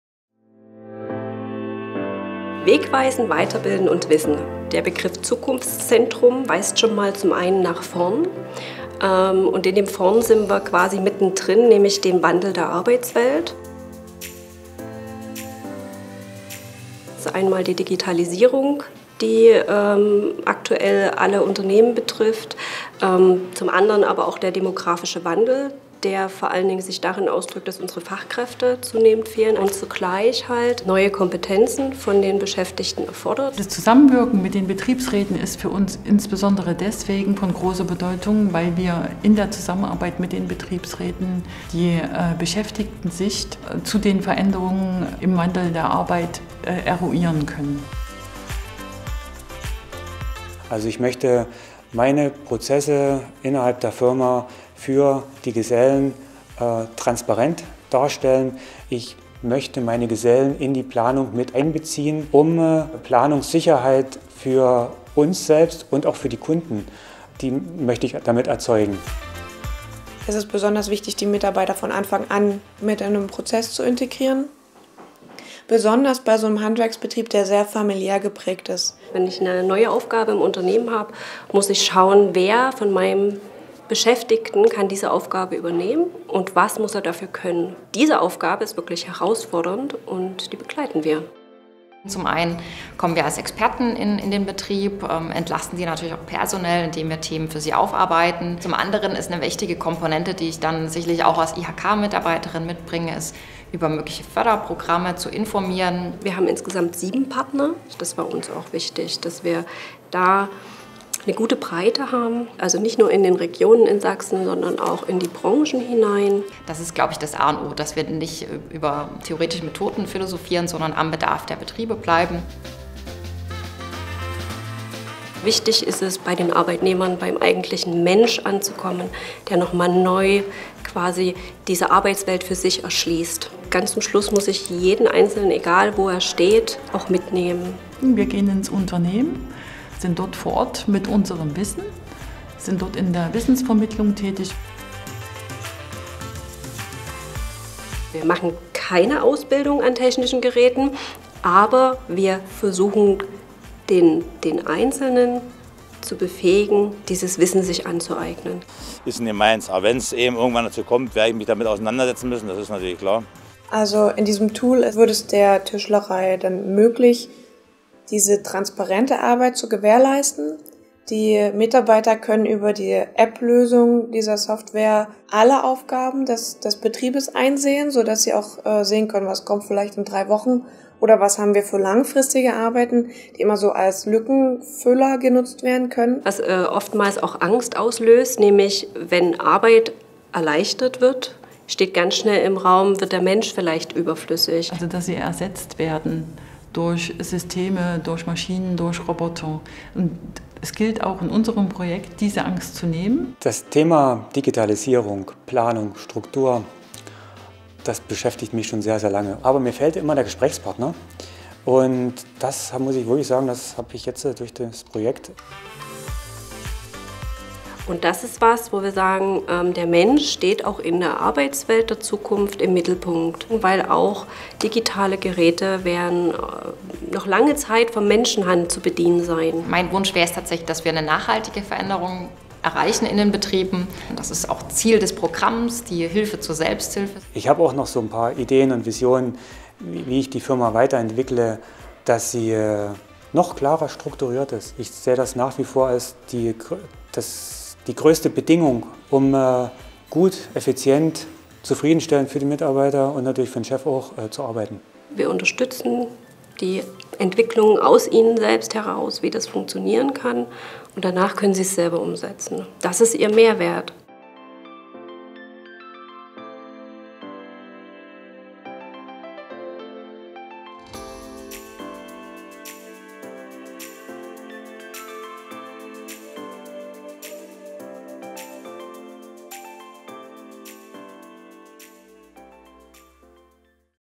Die digitale Arbeitswelt gestalten - fit für die Zukunft
Die Beratung und Qualifizierung von kleinen und mittleren Unternehmen (KMU) im Freistaat ist die Kernaufgaben des vom ESF und BMAS geförderten Projektes „Regionales Zukunftszentrum Arbeit und Qualifizierung neu denken in Sachsen“.

Machen Sie Ihr Unternehmen fit für die Zukunft! Lassen Sie Ihren konkreten Bedarf im Rahmen einer kostenfreien Lotsenberatung ermitteln. Nutzen Sie anschließend unsere Zukunftsberatung, um gemeinsam mit Ihren Beschäftigten passgenaue Lösungen zu entwickeln und umzusetzen.